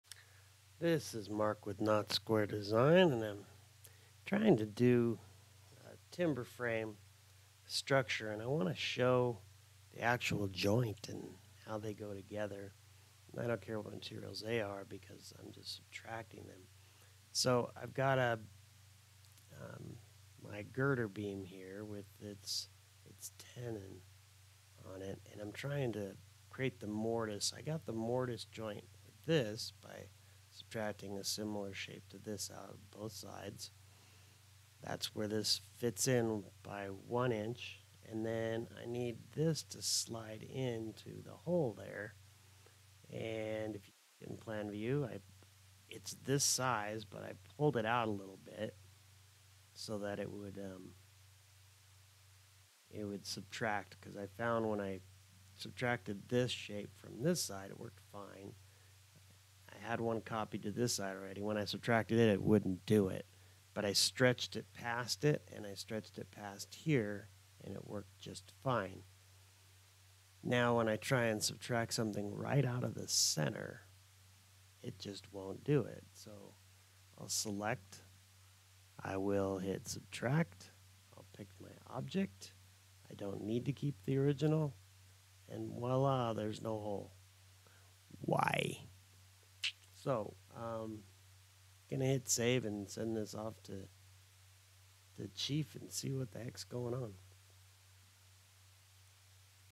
Why Won't This Boolean Command Work
I am trying to subtract this concrete "solid" from the post. The post is a solid as well and I need the mortise for the tenon to insert into. There is another concrete solid in the background in the video. Copies of this were used to subtract the "shoulders" in the post. The left side subtracted flush. On the right, I had to stretch the solid past the surface of the post to get it to work. I have stretched this new concrete block out past the hole I need, but can't on all sides or I will end up with a hole I will need to make patches to get to the hole that just fits the tenon coming out of the girder waiting to the right.